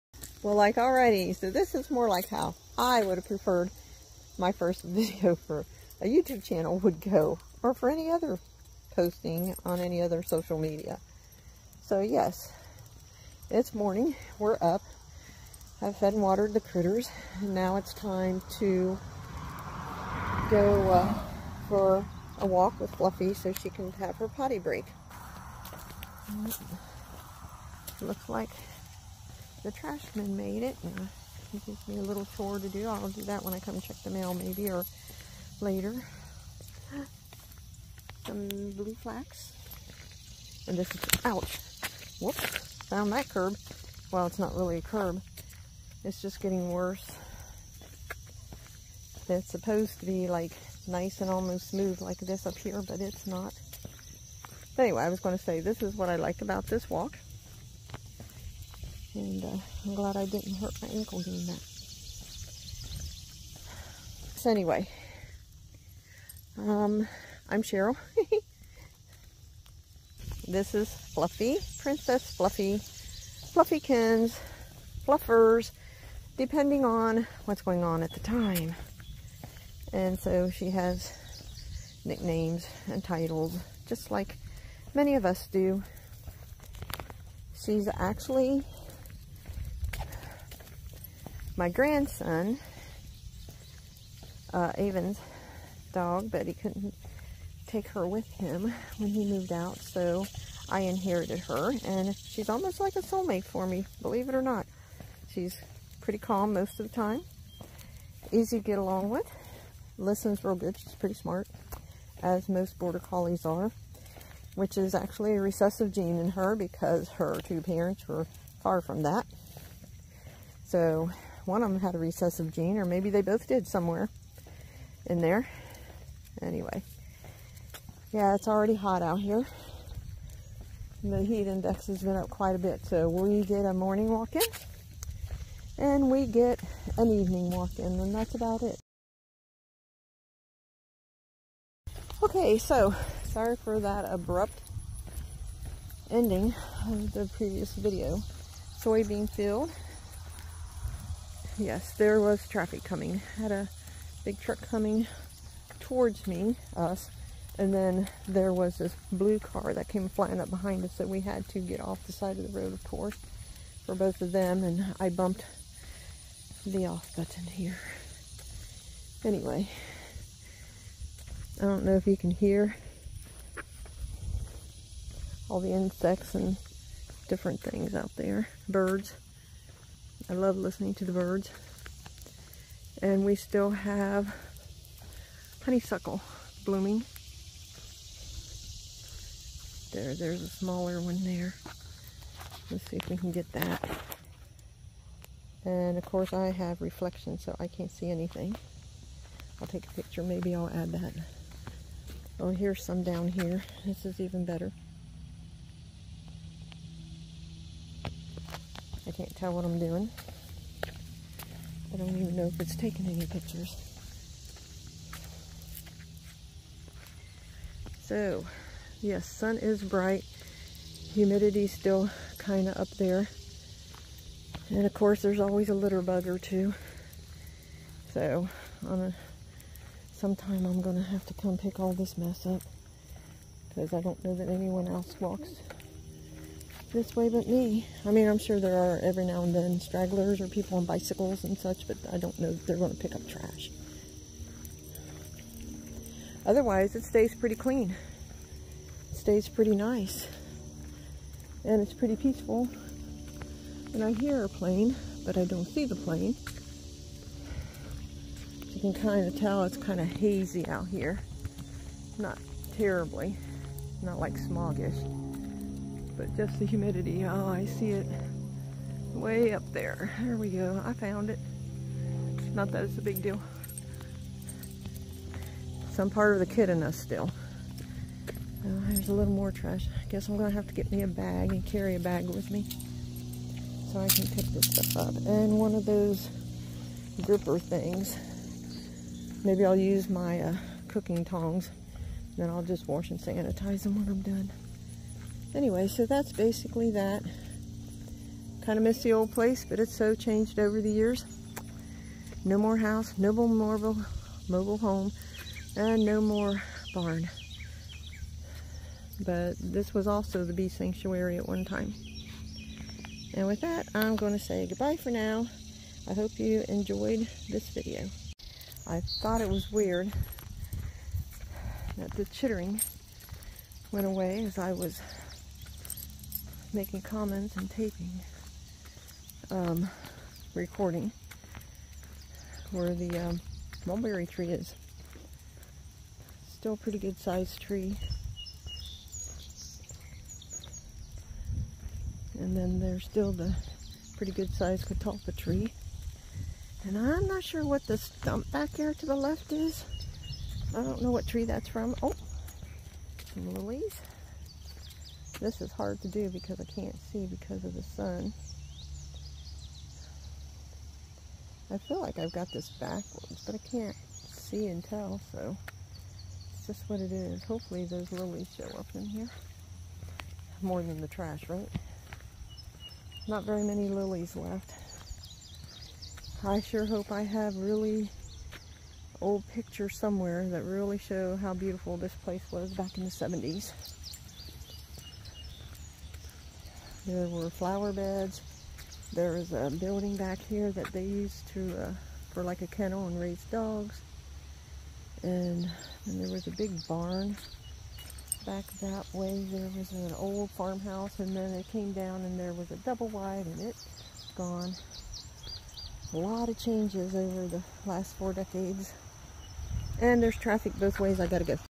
My 1st Original Introduction of myself and Fluffie 07.22.22
Intro of myself and Fluffie. Our routine walk, the neighbor's bee polinating field, and then down to grandma's old place.